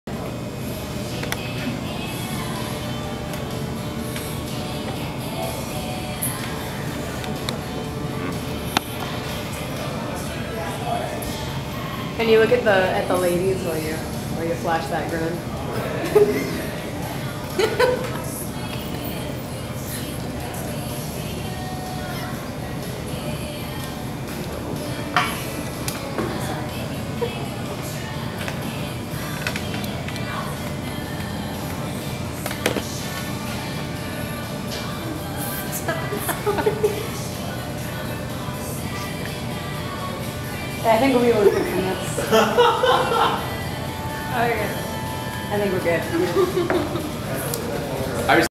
GWS Outtakes #3 - Creeper Smiles
Zoran shows us his different attempts at a creeper smile in this outtake of filming from this weekend (9/2/12)